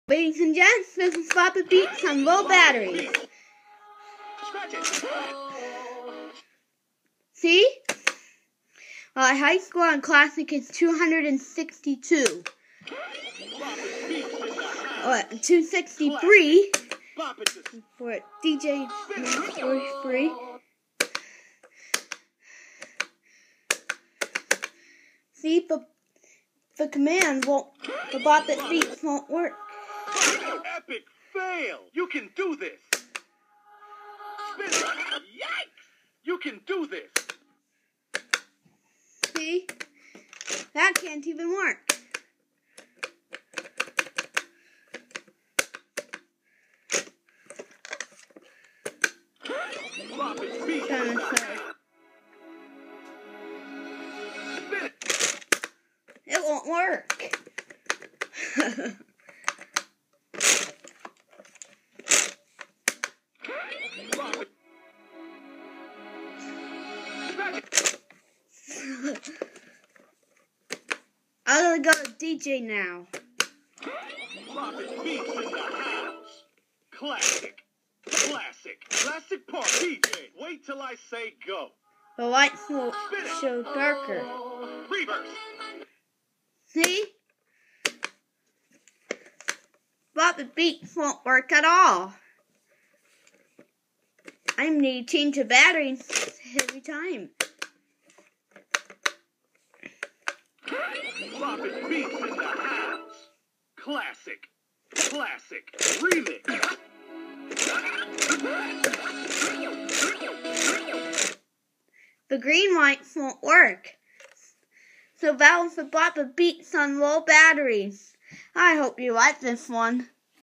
Bop It Beats Low Batteries!
My high score is 263 on Classic and 143 on DJ. But the green lights won't work at all! It is 3% battery remaining. I hope I can change batteries!